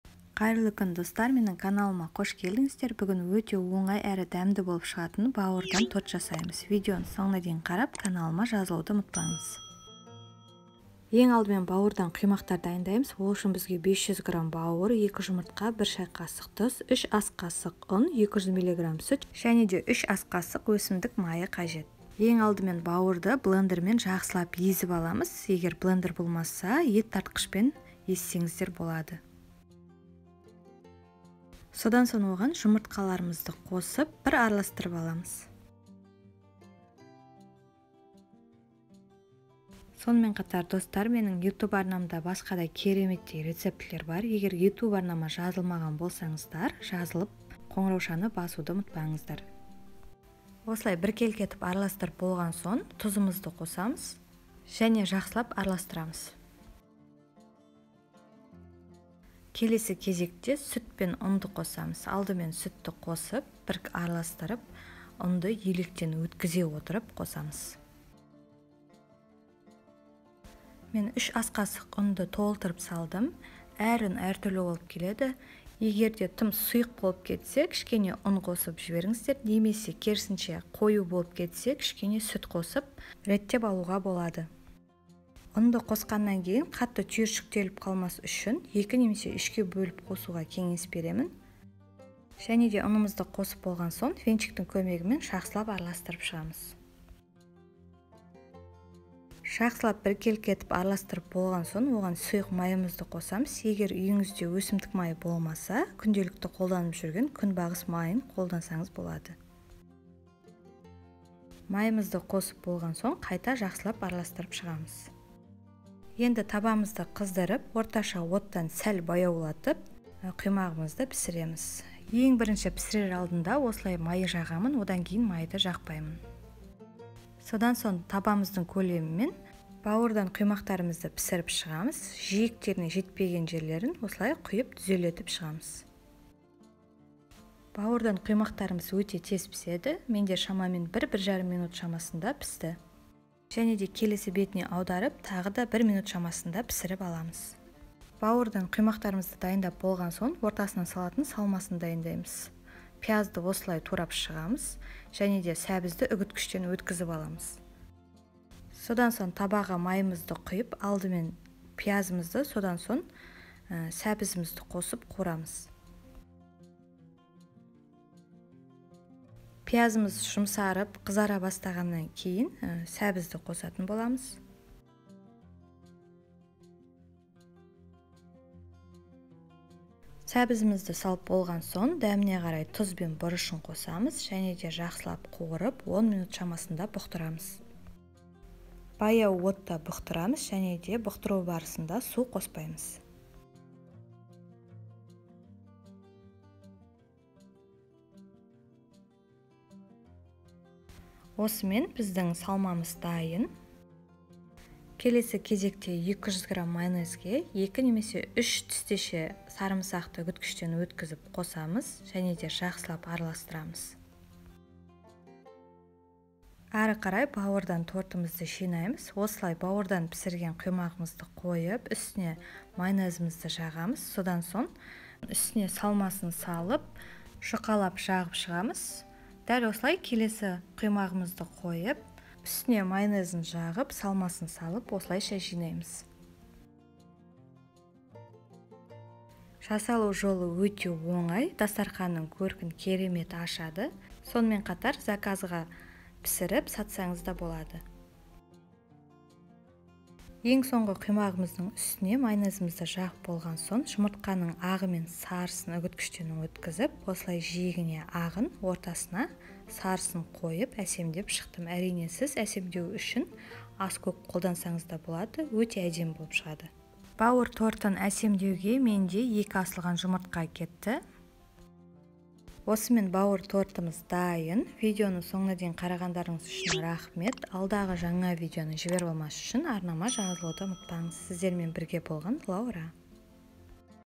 Бауыр торты. Печеночный торт. Бауырдан торт жасау.
Сәлем достар! Менің каналыма қош келдіңіздер! Бүгін Сіздерге Балды цифра тортының рецебімен бөлісемін. Алда жаңа видеоларды жіберіп алмас үшін арнама жазылуды ұмытпаңыз💐

Қажетті заттар:

500 гр бауыр / печень 500 гр
200 мл сүт / молоко 200 мл
3 ас қ. ұн / мука 3 ст л.
3 ас қ. Өсімдік майы / 3 ст л. растительного масла
1 ш.қ тұз / 1 ч.л. соли

Салмасы/ начинка:

2 шт сәбіз / морковь 2 шт
2 шт пияз / лук 2 шт
Татымына қарай тұз, бұрыш / соль и перец по вкусу
2-3 тістеше сарымсақ / 2-3 зубчика чеснока